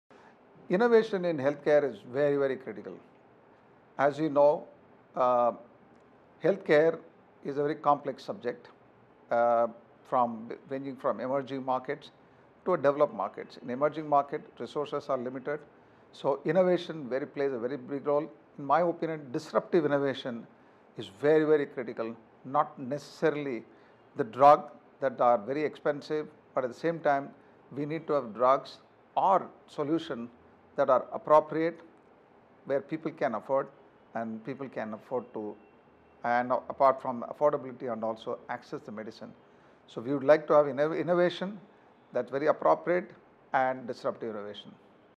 Annaswamy Vaidheesh, VP, South Asia & MD, GSK Pharmaceuticals Ltd., Mumbai
World IP Day Message: Disruptive innovation in healthcare is very critical.